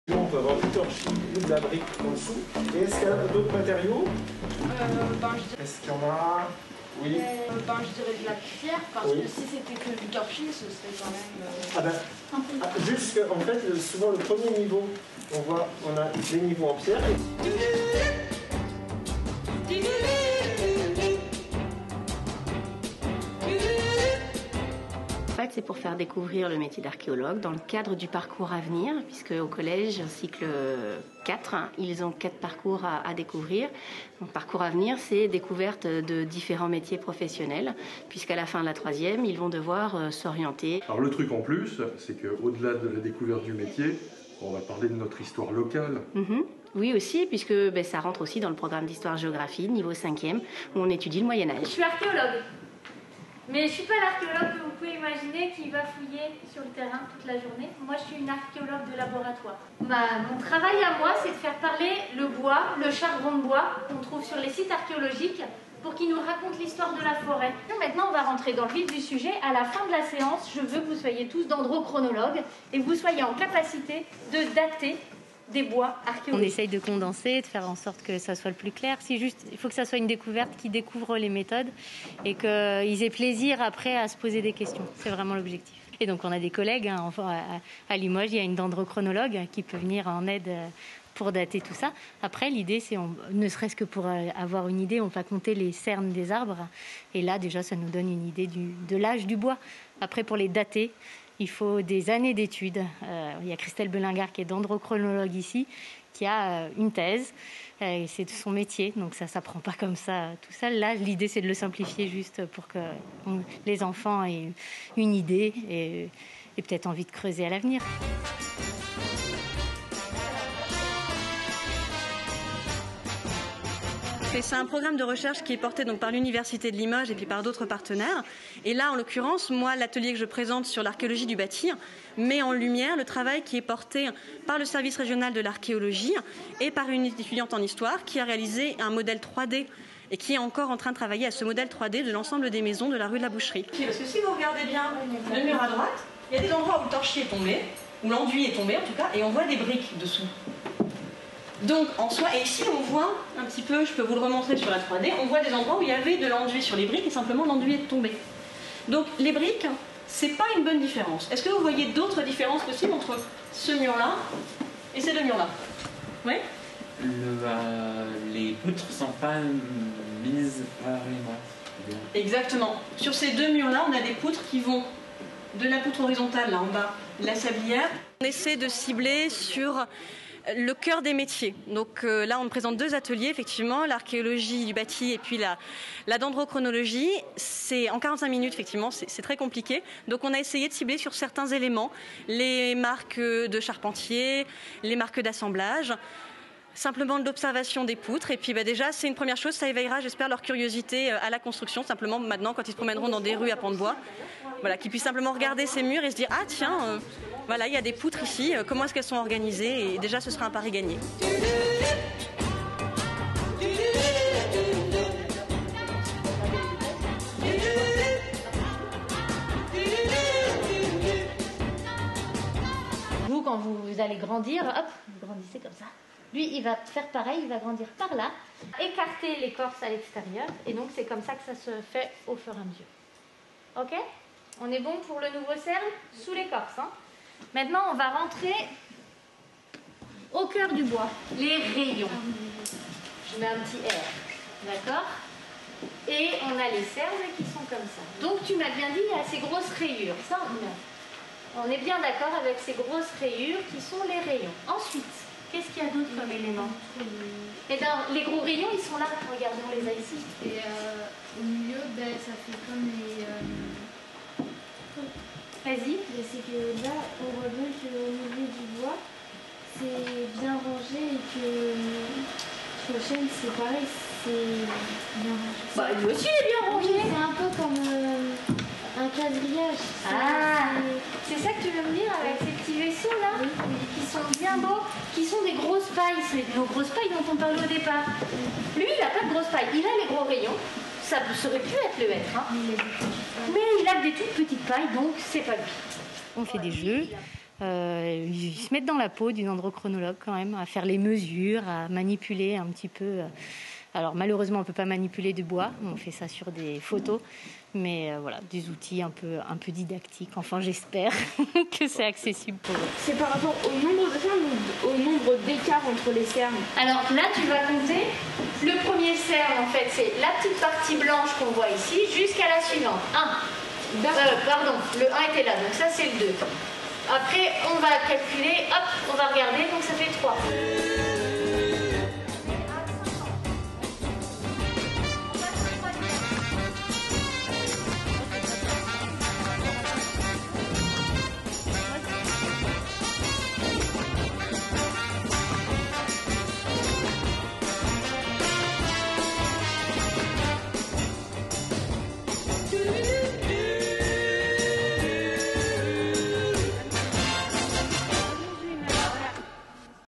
Du bois pour Limoges #03
Du bois pour Limoges #03 Une animation pédagogique est proposée dans le cadre du programme de recherche lancé l'an passé autour de l'origine du bois utilisé à Limoges. Bois de construction, de chauffage aussi. Les collégiens découvrent différentes facettes du métier d'archéologue au passage.